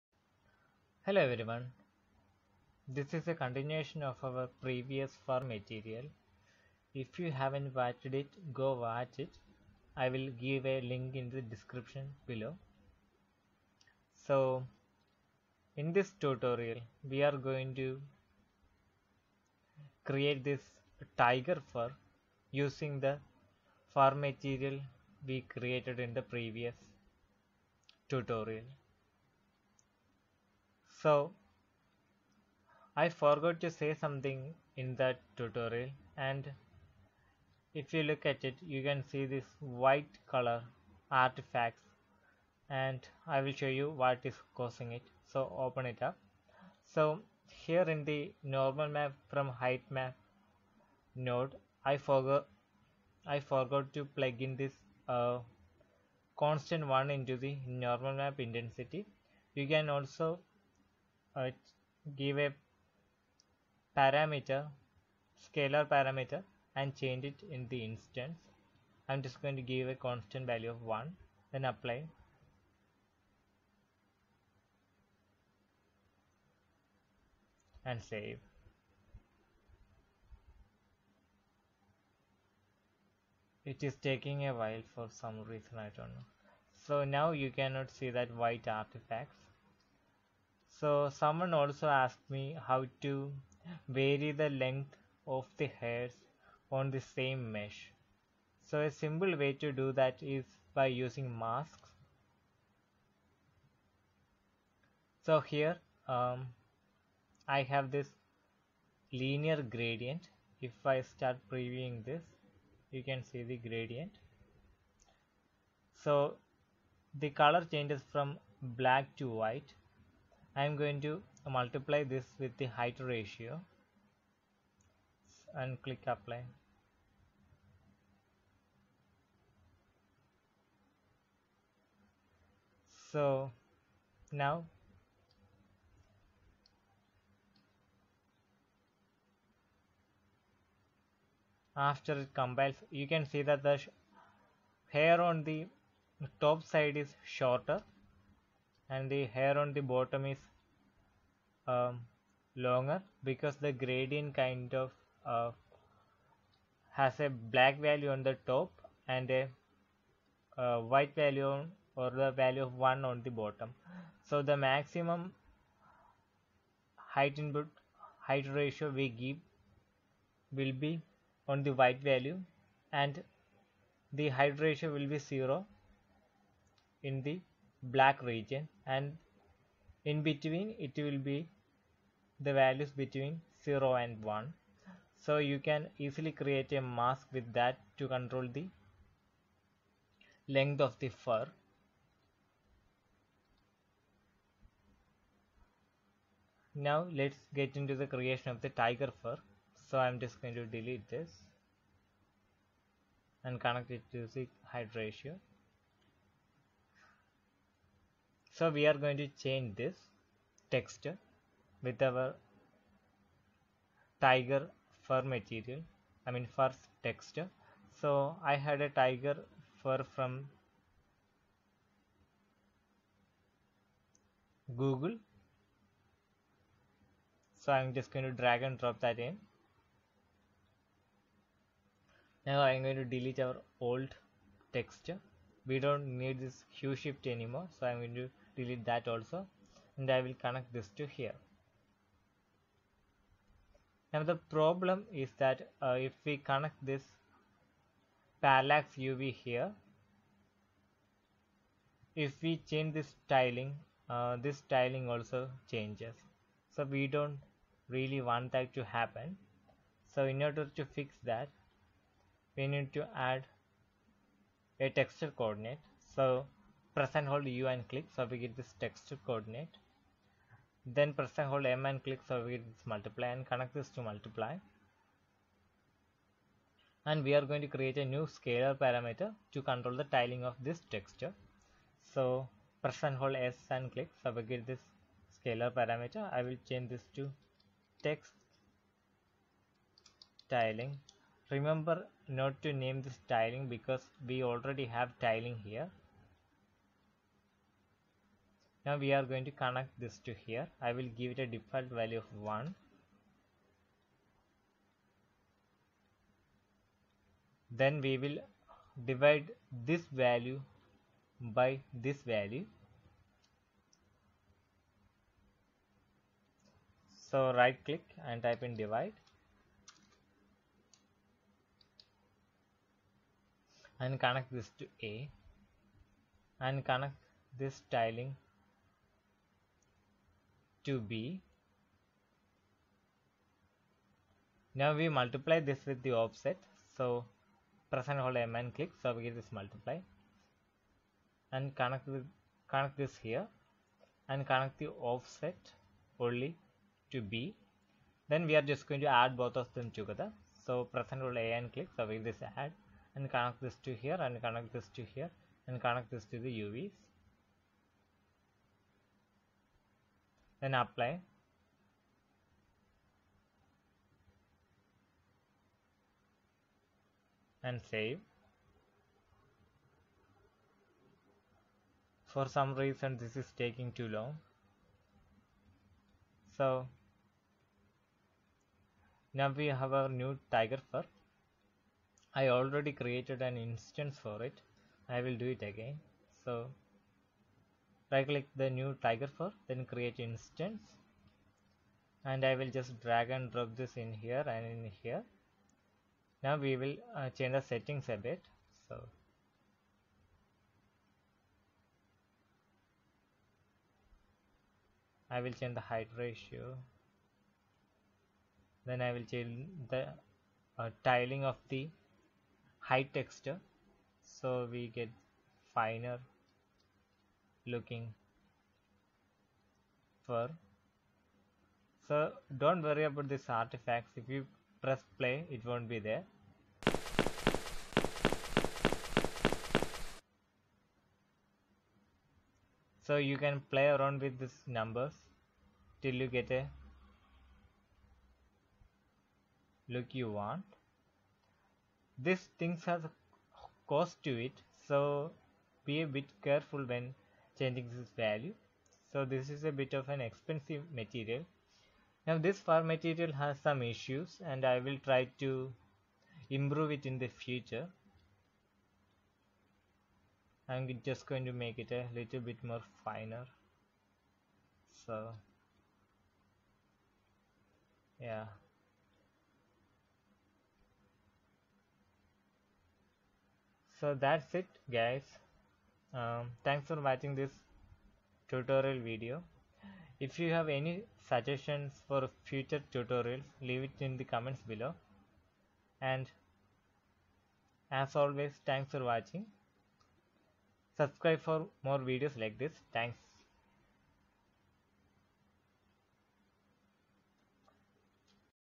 [UE4]-Animal Fur- Material Tutorial
Hello everyone, in this tutorial I will show you how to make a animal\tiger fur material from the fur material we created in the previous video.You can create any animal fur with it and I will show a way to control the length of the fur on the same material,If you enjoyed leave a like and subscribe.